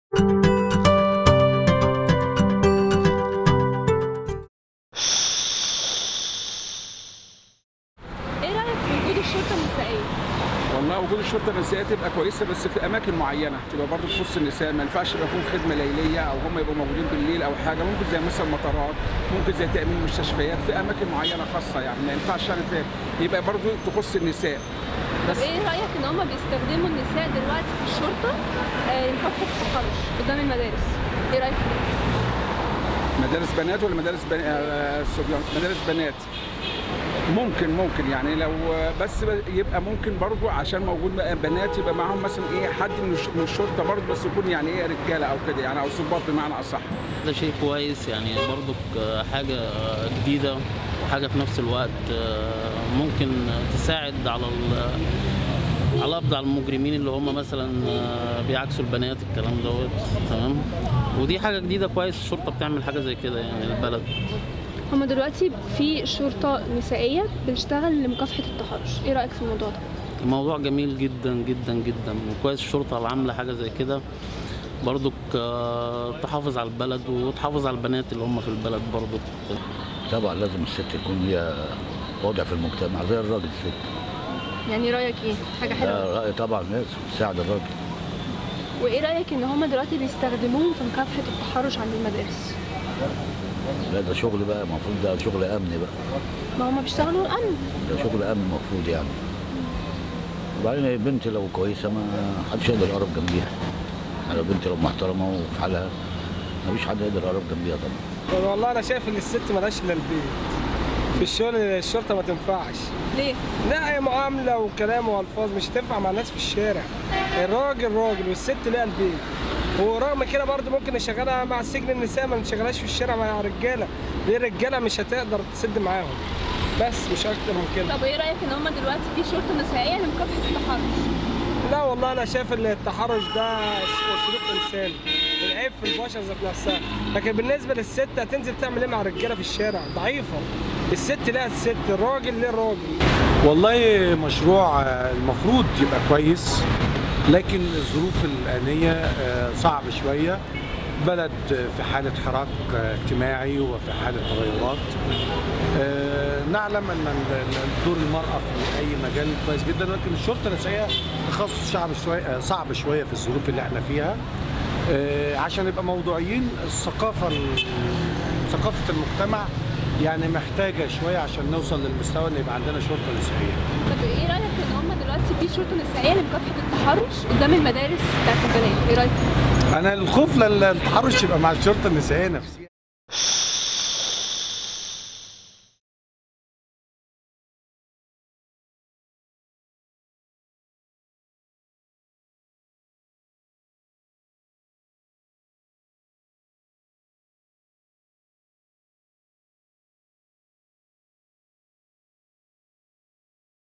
رأي الشارع في الشرطة النسائية
تقرير: عمر ثابت، ميرنا جلال